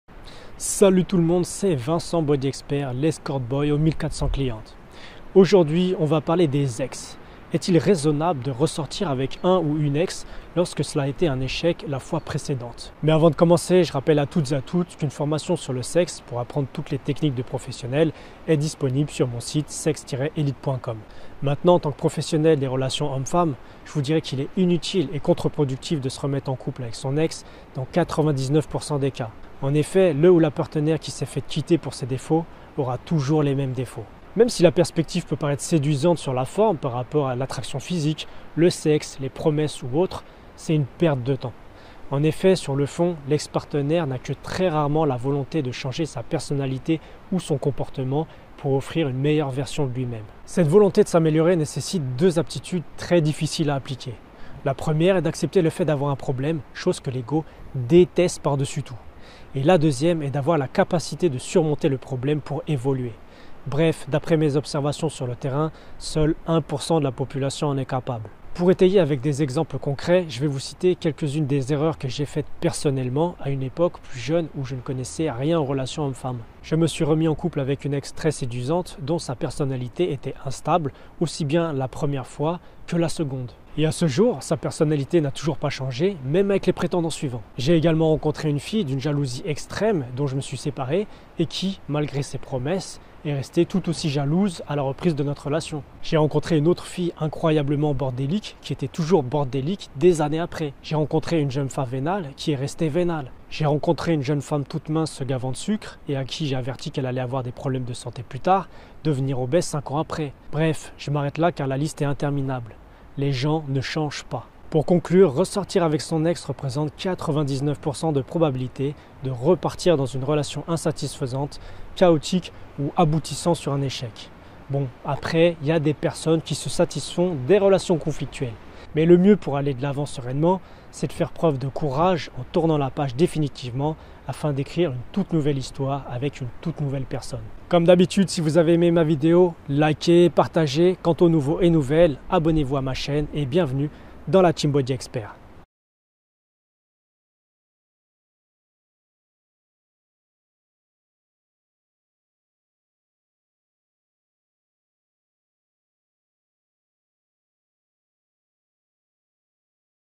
Ressortir avec son ex : bonne ou mauvais idée ?
Pour de nombreuses personnes, l'idée de ressortir avec son ex peut paraître séduisante. Alors voici mes conseils pour éviter certaines erreurs.